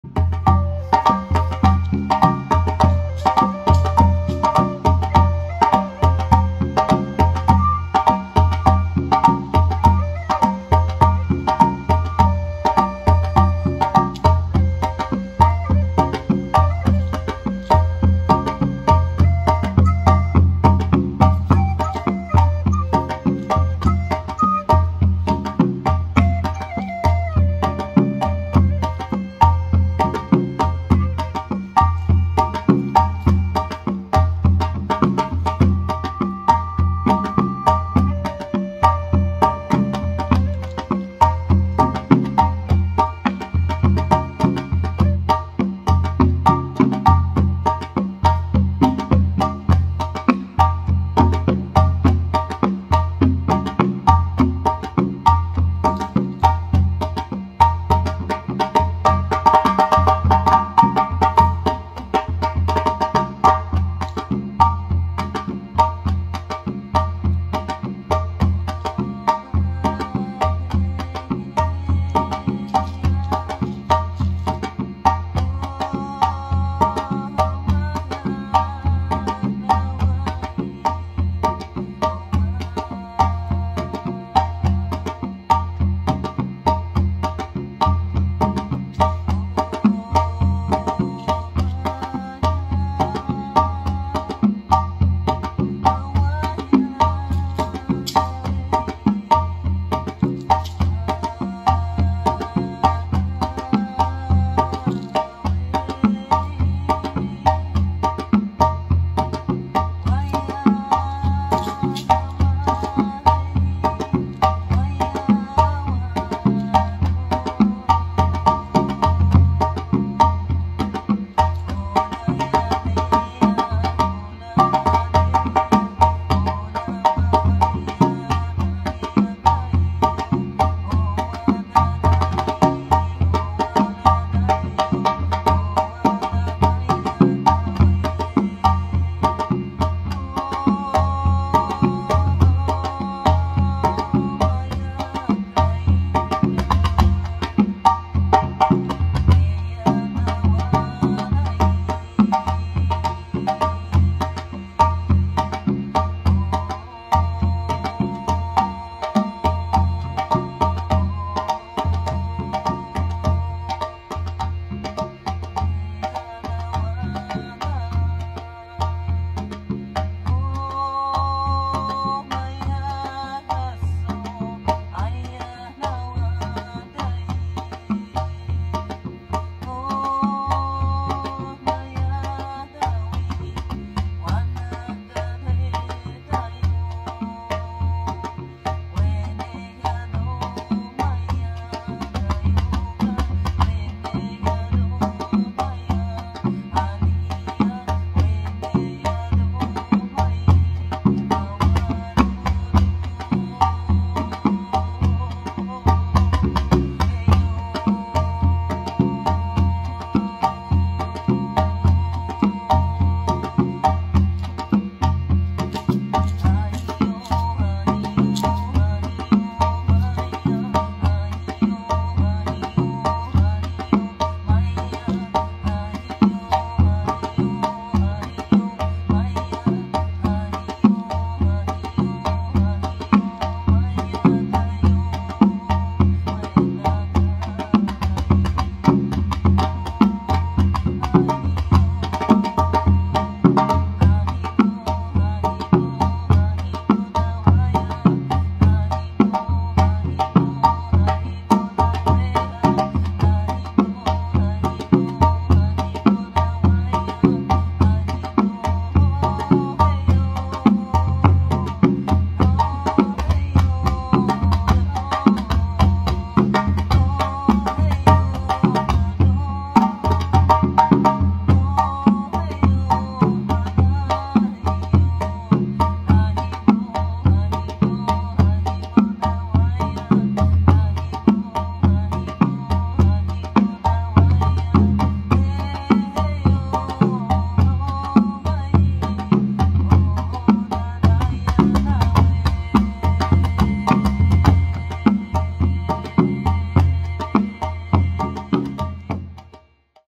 Forest bathing sound medicine
Richard McMullin jembe drum
John Nixon dundun set
Fany Bah voice and flute
.Come come , join us into the heart of the Forest. Sit and allow to soften. Close your eyes. Connect to your heart beat and connecting that beat with the rhythm of our drums. Gentle breeze is bringing the scent of the fresh forest leaves and moss. Branches are dancing above ancient dance of connection. The wisdom of the forest is rejuvenating our hearts, sparkling passion and unconditional Love for all Mother Earth Creations. Gratitude for being part of our journey ❤️

Connect to Sun Drum Forest:
 ❤ Peace and blessings to you 🙏
May all beings be free and happy 🙏